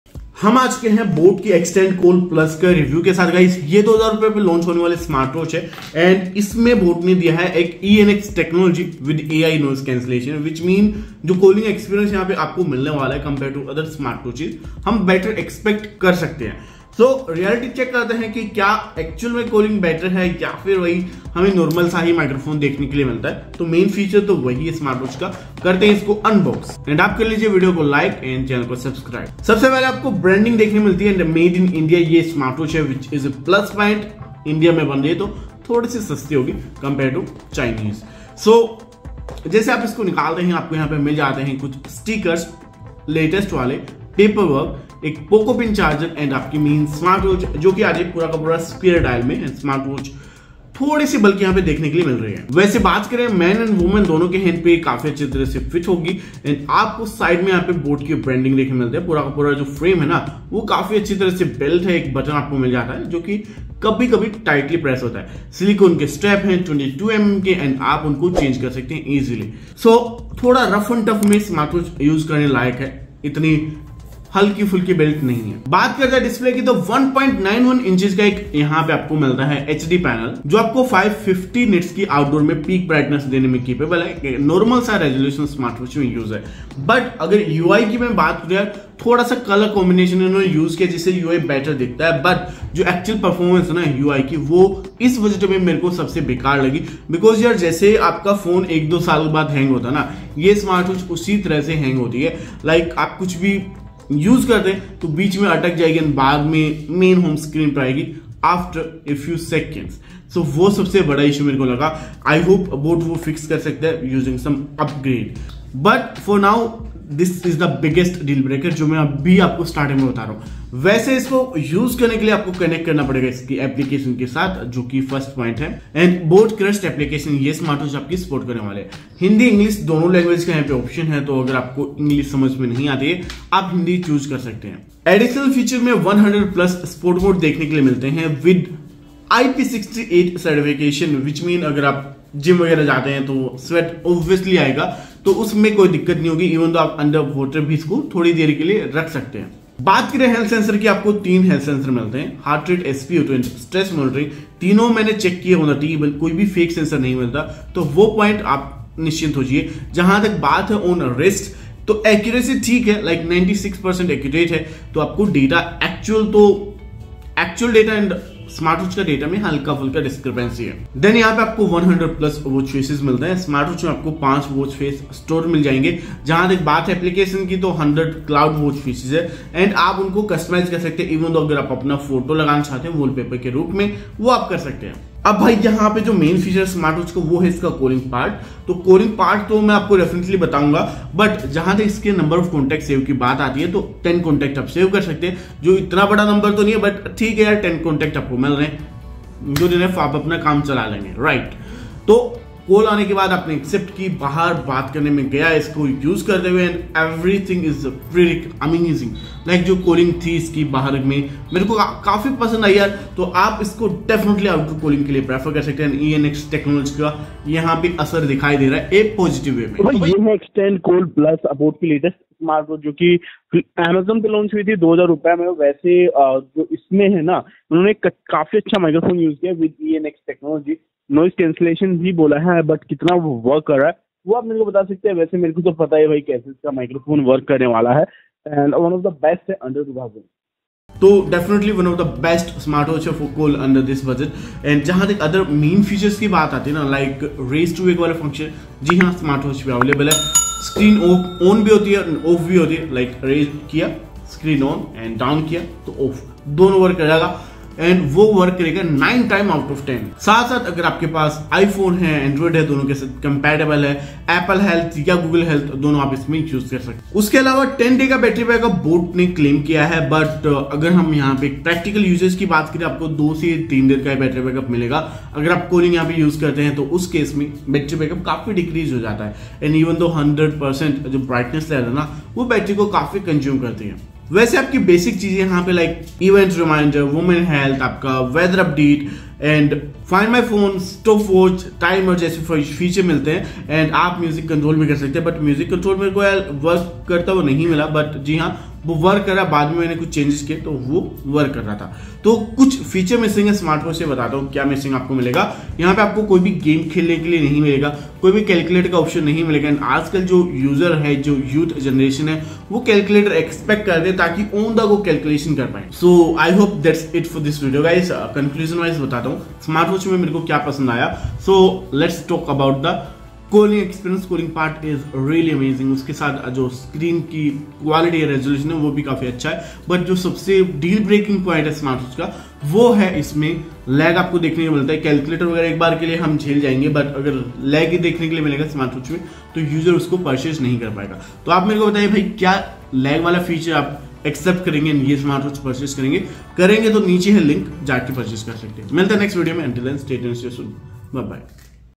Boat xtend call plus smart watch unboxing || Best smartwatch under 2000
boAt xtend call plus smartwatch unboxing || Best smartwatch under 2000 with ENx.  boat latest smartwatch under 2000 rupees is here with calling function and this has enx technology which mean AI noise cancellation for call mean better calling under 2000 rupees. This is the best smartwatch under 2000 rupees for calling.

boat xtend call plus